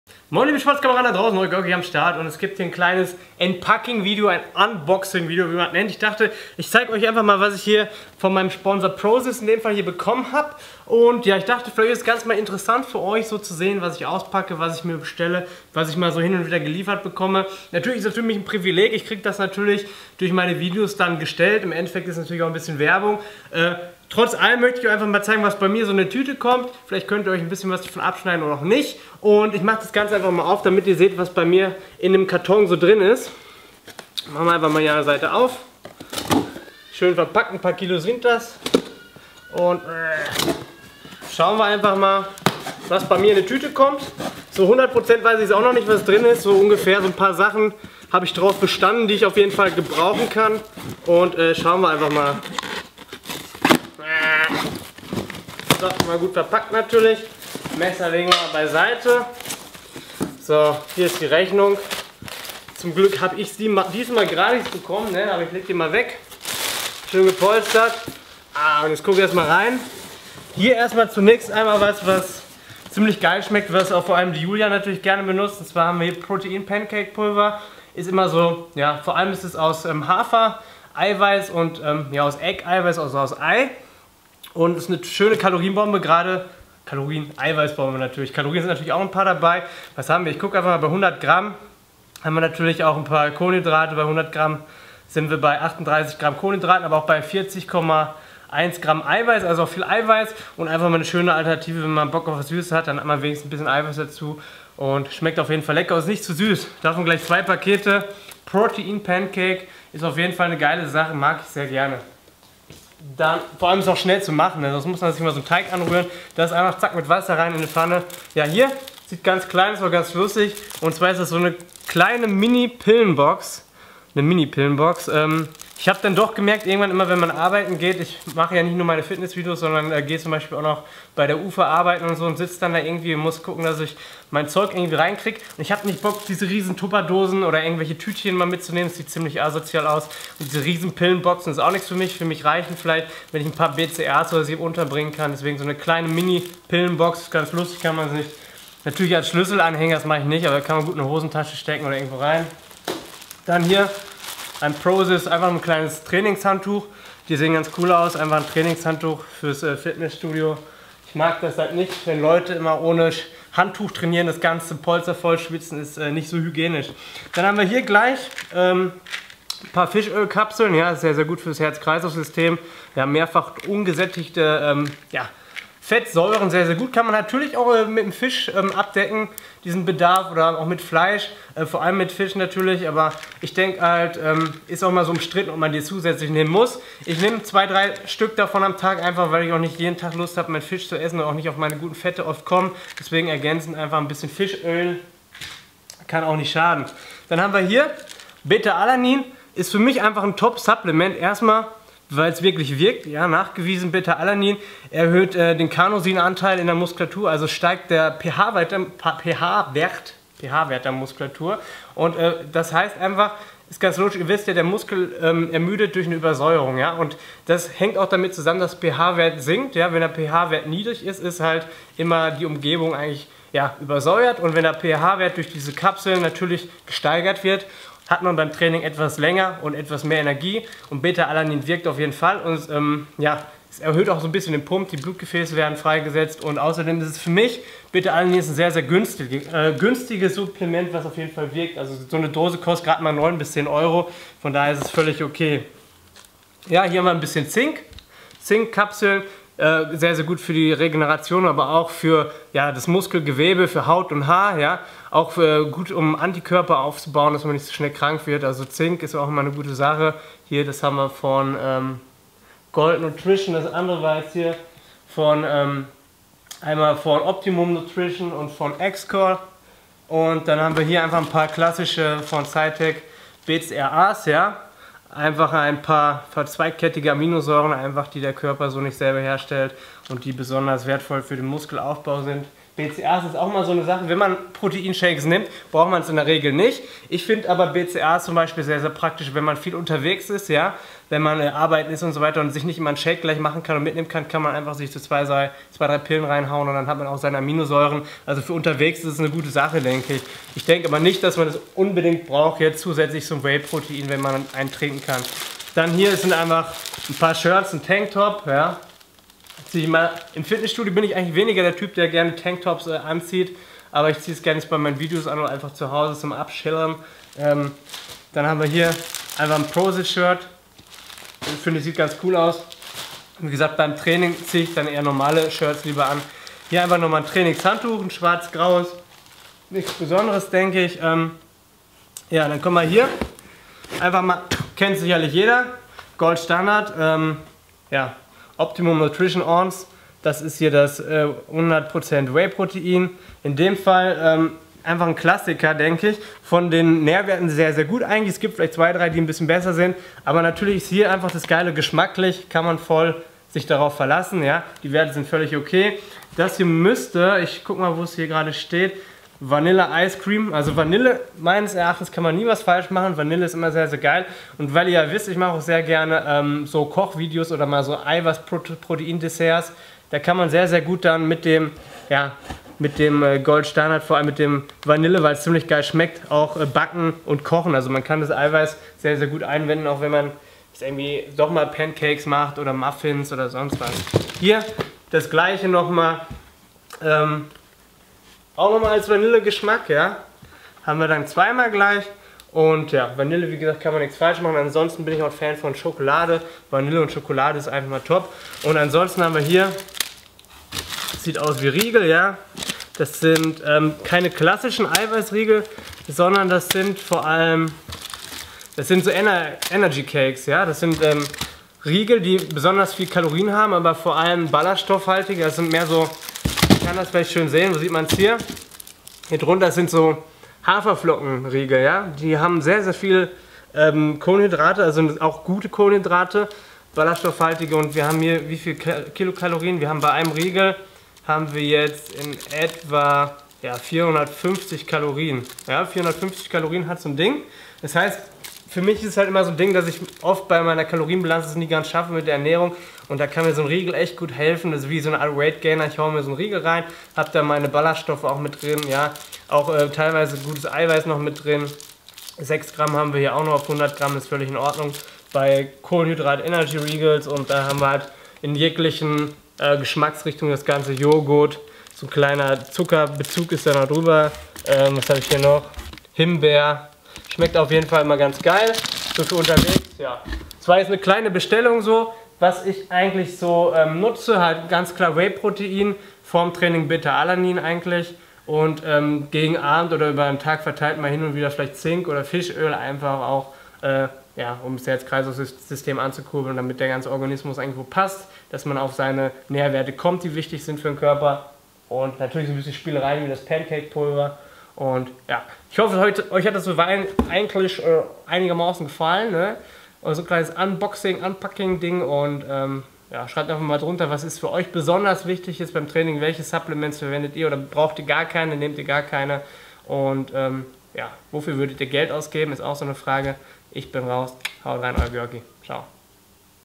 Supplement Unboxing
Ich habe Post von Prozis bekommen! Was habe ich mir so alles an Supplements bestellt? Neben dem Whey Protein gibt es noch so einiges anderes was ich mir bestellt habe, ich erzähle euch ein wenig was darüber!